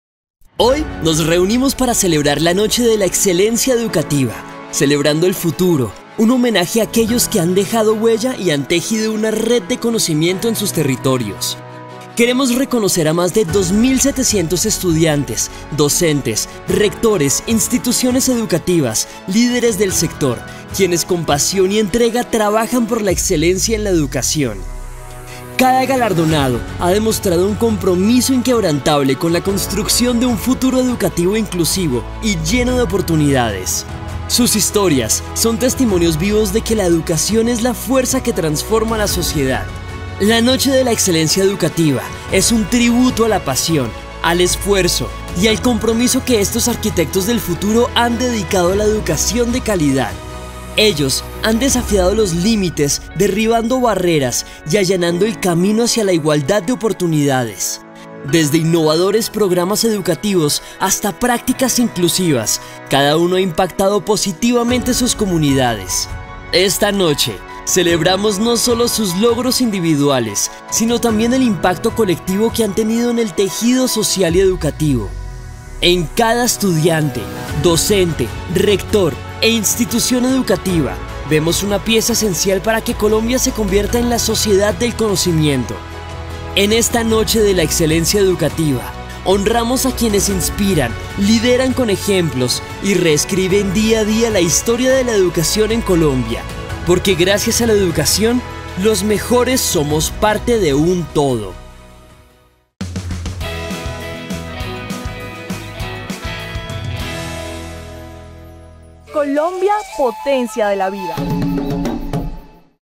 Galardonados de la Noche de la Excelencia Educativa
Nos reunimos para celebrar la NocheDeLaExcelencia2023, un homenaje a aquellos que han dejado huella en sus territorios. Queremos reconocer a más de 2.700 estudiantes, docentes, IE, líderes del sector quienes con dedicación y entrega trabajan por la excelencia en la educación.